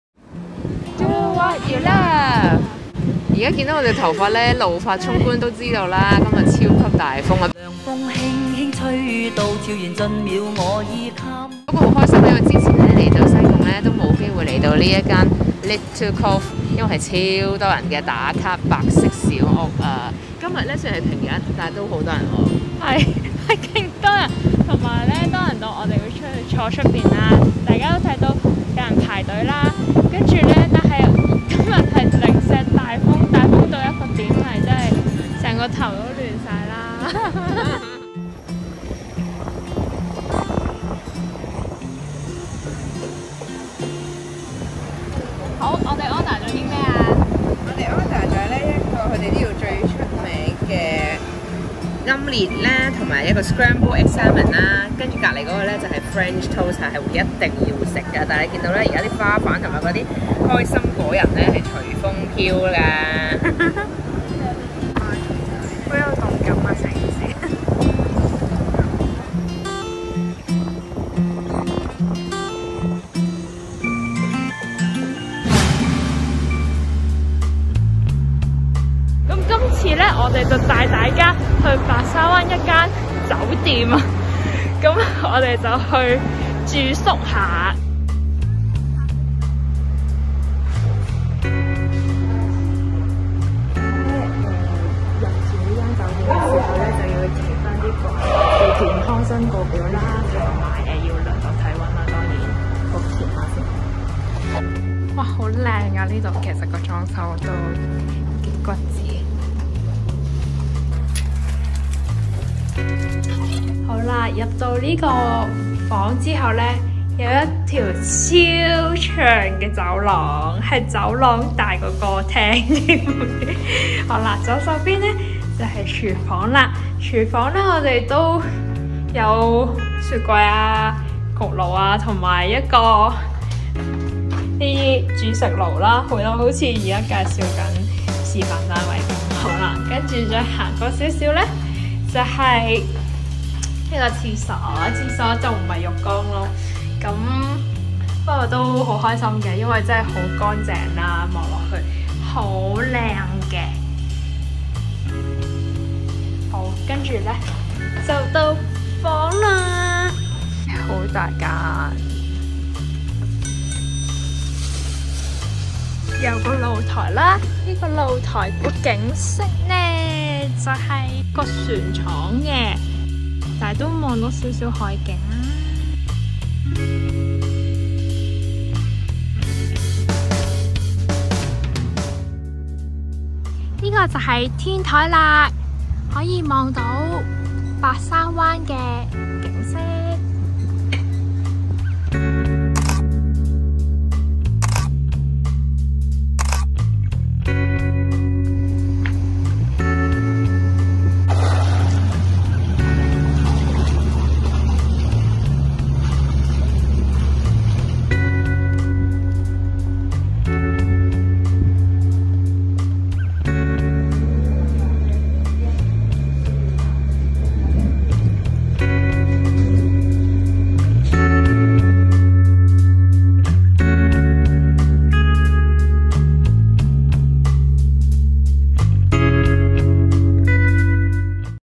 ［慢活房子🏠漫遊推介- 去間 cafe + 去個 staycation@西貢］
西貢最近多咗唔少嘅Cafe進駐，其中呢一間 littlecoveespresso就係西貢打卡cafe之冠，平日都要排長龍，可想而之，佢哋嘅知名度係幾咁勁。嘢食方面，都夠哂fresh，有好出名嘅French Toast，真心好味道🥰
Little Cove Espresso
Address: Siu Yat Building Block A, Shop 1+2, G/F, Sai Kung Hoi Pong Square, Sai Kung

最近興Staycation，小編們走在潮流尖啄，當然都唔會執輸啦！今次我哋就去到西貢白沙灣酒店 thepier ，環境同氣氛都相當唔錯，由其天台位更係打卡熱點，180度白沙灣靚景，平日嚟一個悠閒小旅行！差電差電！不錯吖😘
The Pier Hotel
Address: No.9 Pak Sha Wan Street, Sai Kung